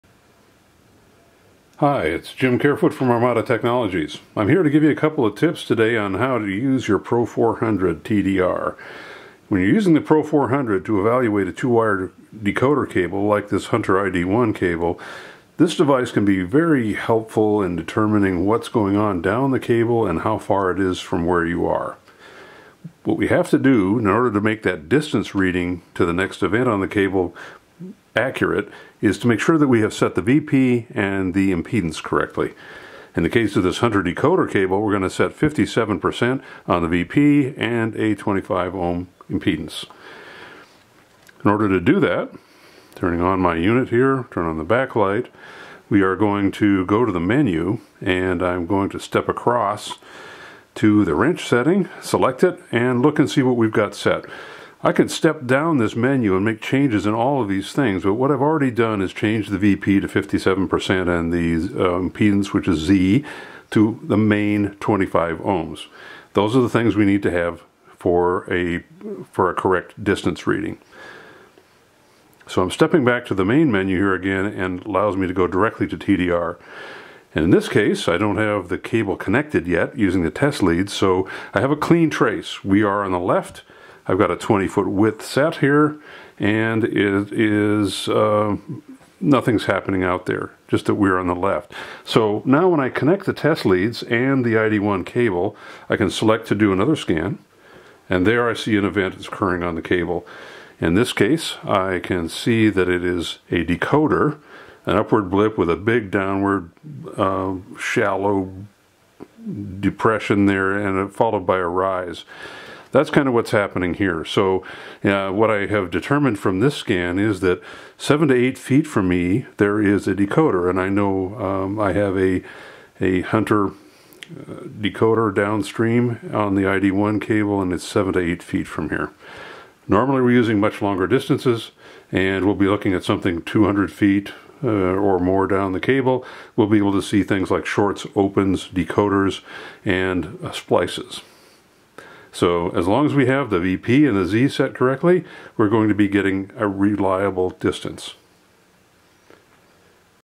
Getting the Pro400 TDR Set Up for 2-Wire decoder troubleshooting application
The two key set-up parameters are Vp (velocity of propagation) and cable impedance in ohms. When correctly set the distance will be most accurate.